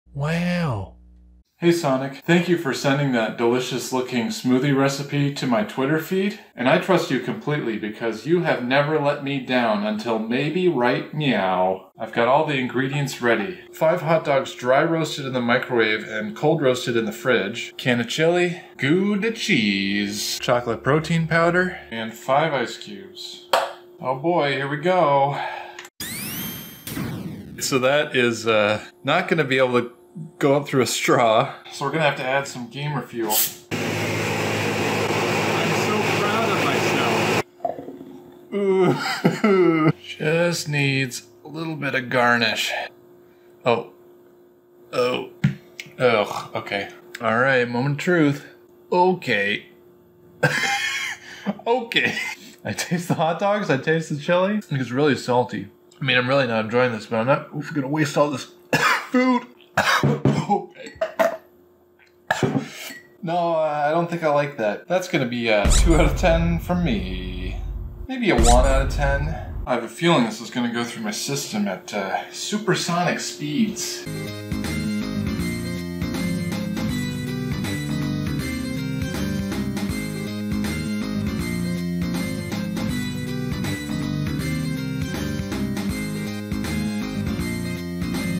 Sonic's Chili Dog Protein Shake - Review
Hi Sonic the Hedgehogs, thank you for all you've done for me over the years, but heck off for what your smoothie recipe has done to me now. I wasn't going to see your new movie, but now I refuse.

I wanted to be ready for Mario and Sonic and Dingus at the Olympic Games. My fitness was at its peak. I could bench press 50 pounds. But now all my lifestyle changes have reversed. Ever since your wretched shake recipe, I've joined Dr. Eggman's side now. He's got omega 3 fatty acid. What do you have. Digestive problems. I mean that's what I have. Thanks to you. Coward.